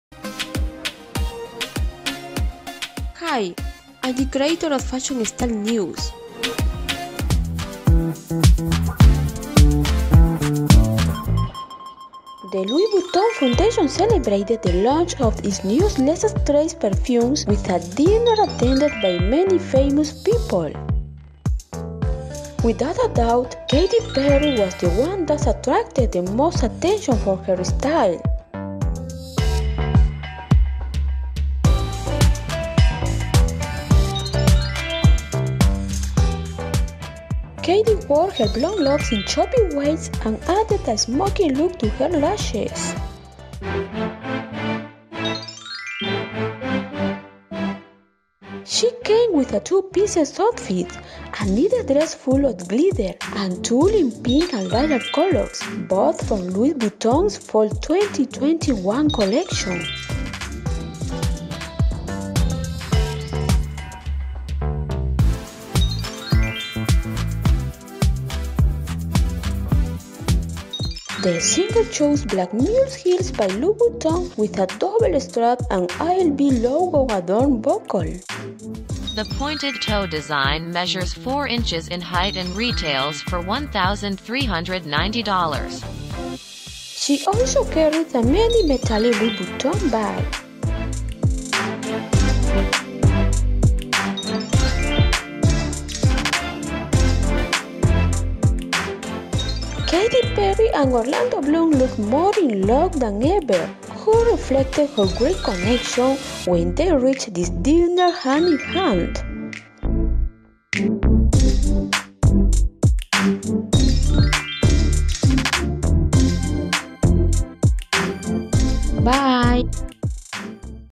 Glamorous Katy Perry in lilac tulle knit dress with sparkles at the Louis Vuitton Les Extraits event
Katy Perry looked glamorous in a sparkly lilac tulle knit dress at Louis Vuitton Les Extraits event. At the presentation of Les Extraits perfumes. Without a doubt, Katy Perry was the one that attracted the most attention for her style. She came with a 2-piece oufit: a knitted dress full of glitter and tulle in pink and lilac colors, both from Louis Vuitton's fall 2021 collection. The singer chose black mules heels. Katy and Orlando Bloom looked more in love than ever.